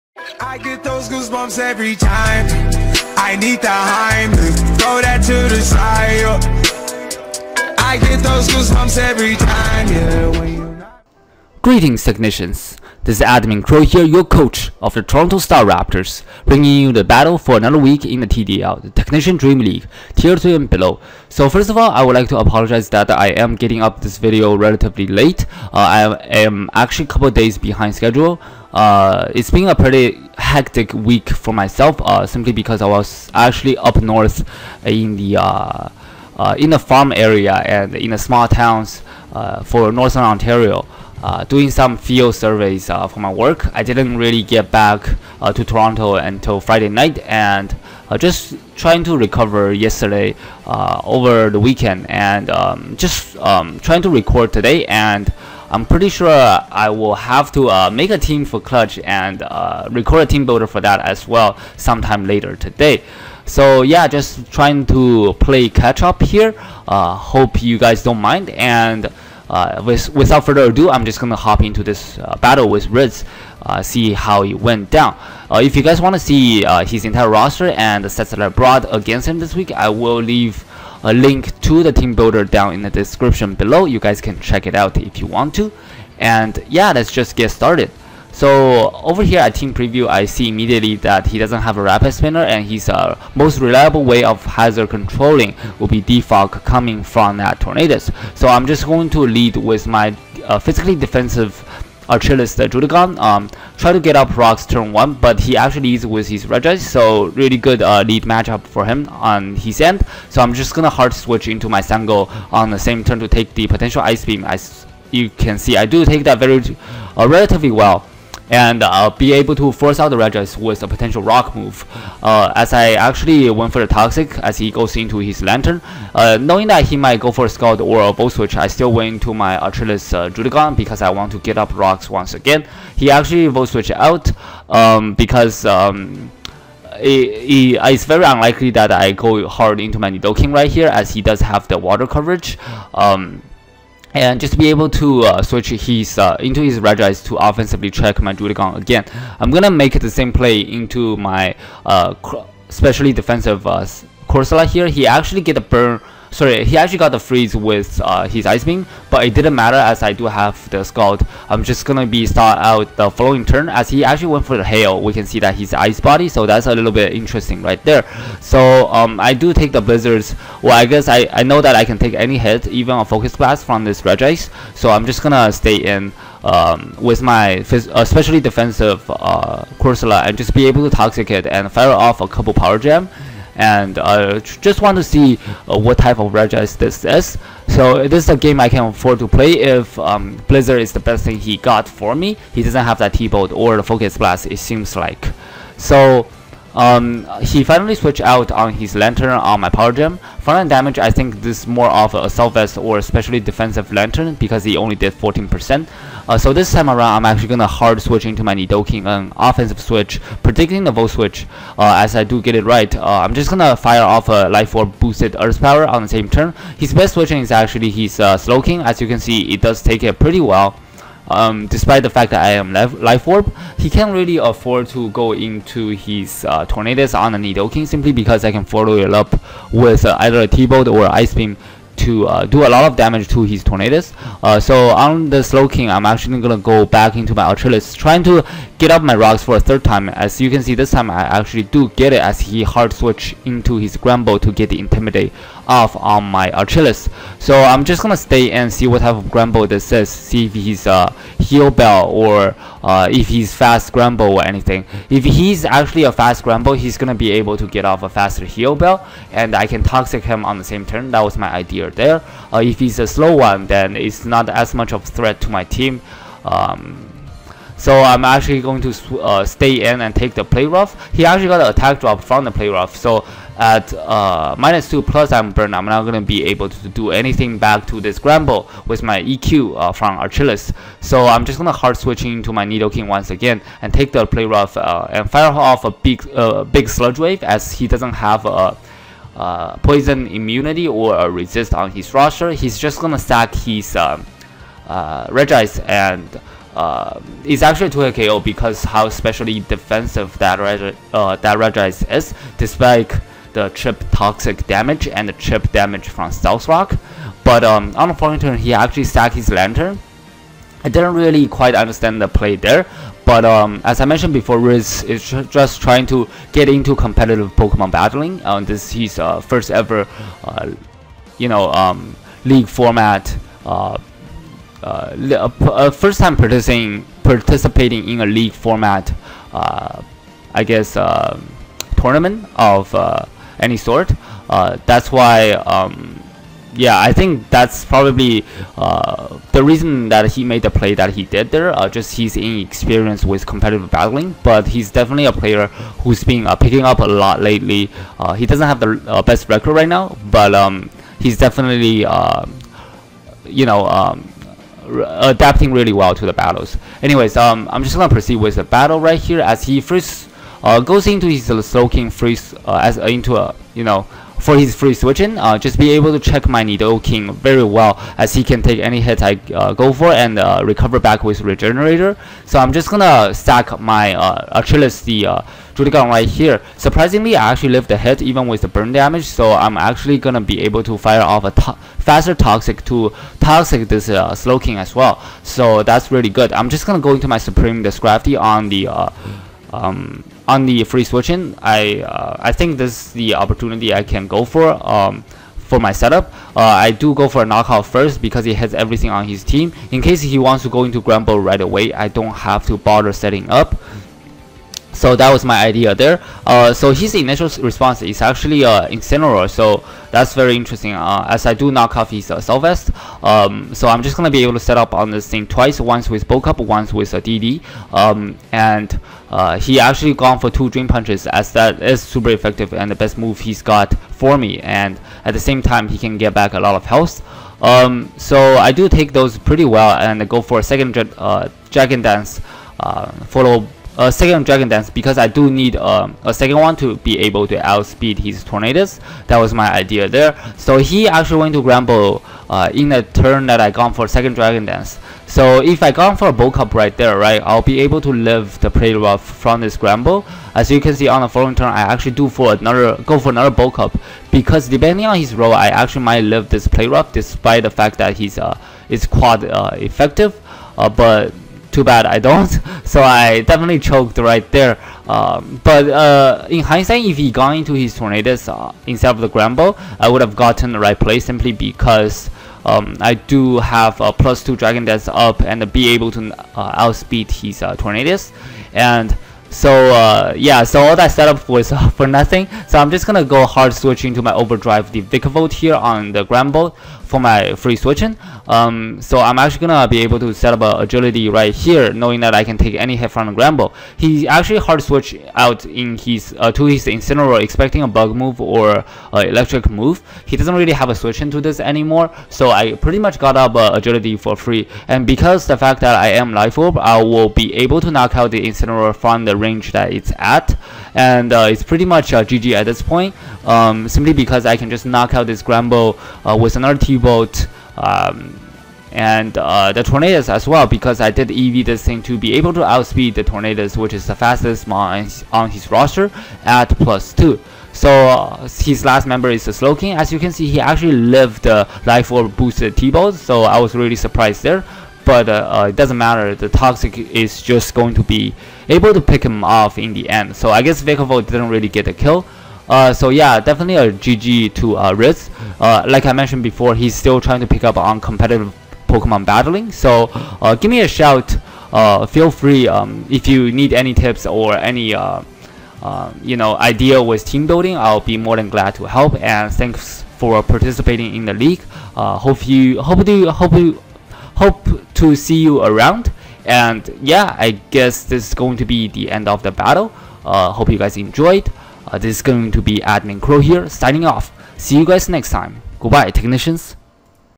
"Frozen AGAIN?!!!!" TDL Off Seaon T3&Below W5 Battle vs Vancouver Spectres
Greetings Techs, we are taking on Riz the coach of the Vancouver Specres this week. Riz is a new coach who's trying to get into competitive battling. He may not have the best record this season but he is definitely adapting fast. Lets see how the battle went down?